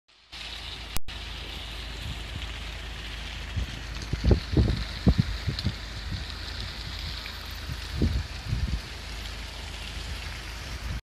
Požari 'divljaju' po Dalmaciji: Evakuirali su dio ljudi na Pagu I Snimio čitatelj
Na otoku Pagu vatrogasci su iz preventivnih razloga privremeno iselili 15-ak ljudi iz kuća. Požar na Viru obuzdan je tek ujutro, a stanovnici su već spakirali kofere i bili spremni na evakuaciju

Imate priču?